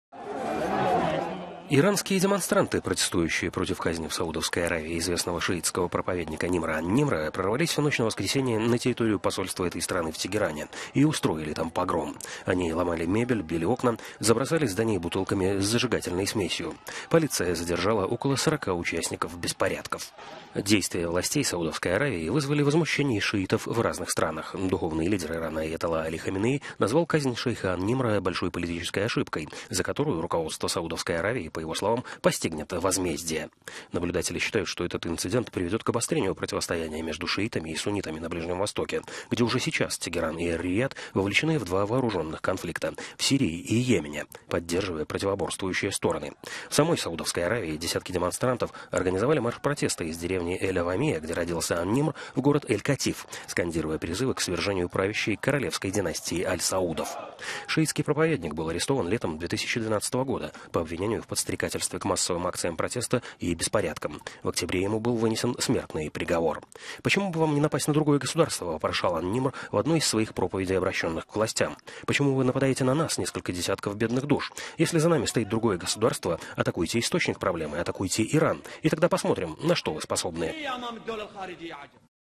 В Тегеране демонстранты устроили погром в посольстве Саудовской Аравии
Иранские демонстранты, протестующие против казни в Саудовской Аравии известного шиитского проповедника Нимра ан-Нимра, прорвались в ночь на воскресенье на территорию посольства этой страны в Тегеране и устроили там погром. Они ломали мебель, били окна, забросали здание бутылками с зажигательной смесью. Полиция задержала около 40 участников беспорядков.

Действия властей Саудовской Аравии вызвали возмущение шиитов в разных странах. Духовный лидер Ирана аятолла Али Хаменеи назвал казнь шейха ан-Нимра большой политической ошибкой, за которую руководство Саудовской Аравии, по его словам, постигнет возмездие.

Наблюдатели считают, что этот инцидент приведет к обострению противостояния между шиитами и суннитами на Ближнем Востоке, где уже сейчас Тегеран и Эр-Рияд вовлечены в два вооруженных конфликта – в Сирии и Йемене, поддерживая противоборствующие стороны.

В самой Саудовской Аравии десятки демонстрантов организовали марш протеста из деревни Эль-Авамия, где родился ан-Нимр, в город Эль-Катиф, скандируя призывы к свержению правящей королевской династии Аль Саудов.

Шиитский проповедник был арестован летом 2012 года по обвинению в подстрекательстве к массовым акциям протеста и беспорядкам. В октябре 2014-го ему был вынесен смертный приговор. 2 января ан-Нимра и еще 46 человек, обвиненных в терроризме, казнили.

“Почему бы вам не напасть на другое государство? – вопрошал ан-Нимр в одной из своих проповедей, обращенных к властям. – Почему вы нападаете на нас, несколько десятков бедных душ? Если за нами стоит другое государство, атакуйте источник проблемы – нападите на Иран. Если вы выступаете против Ирана, атакуйте его, и тогда посмотрим, на что вы способны”.